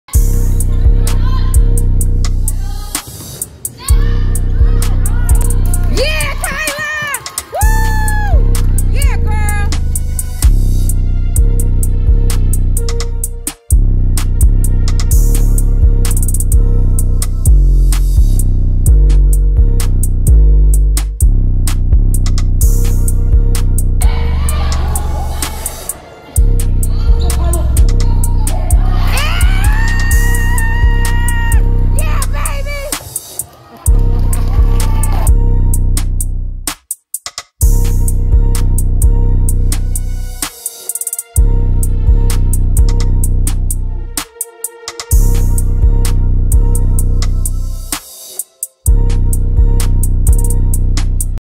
Volleyball Dink Shots, Kyla Davis
Kyla is improving her skill set, by adding the dink shot to her repertoire